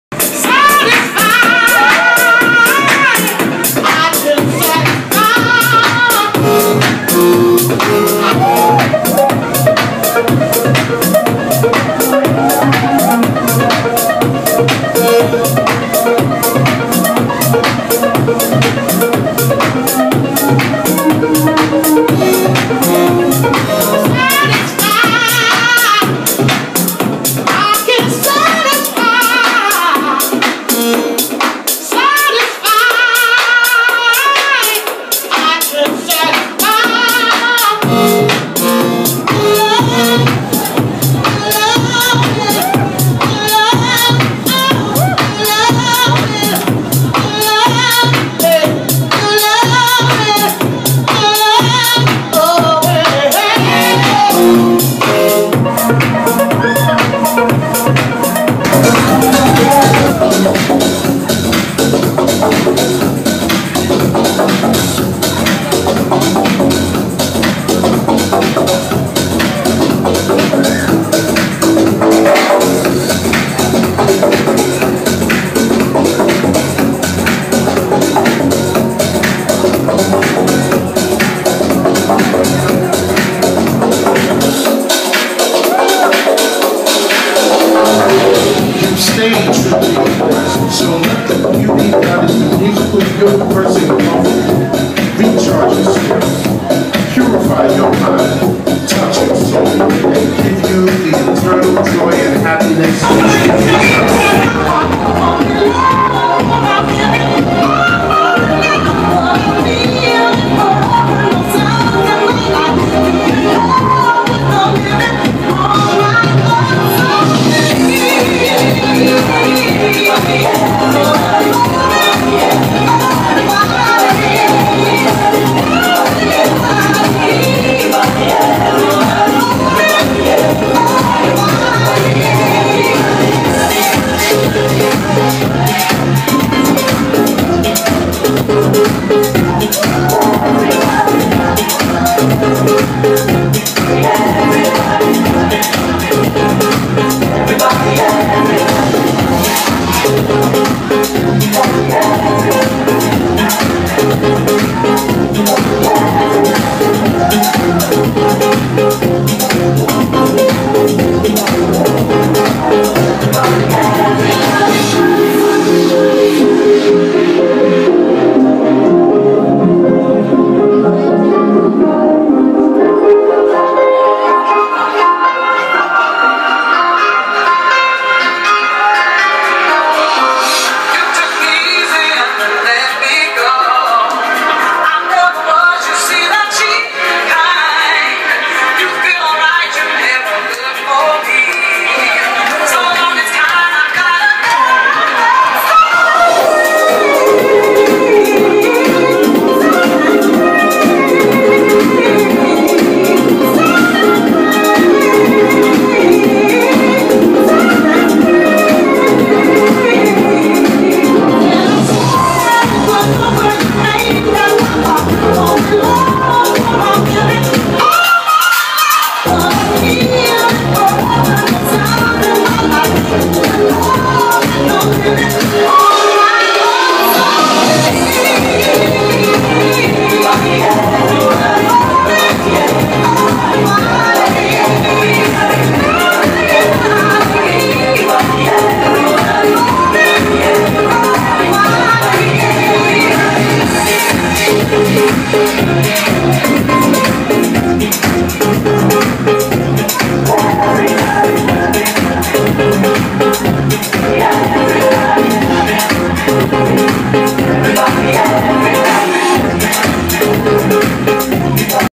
MASANORI MORITA Djing @ B1 / 2013. 09. 15 sun
playing as a secret dj @B1, surprise guest dj~!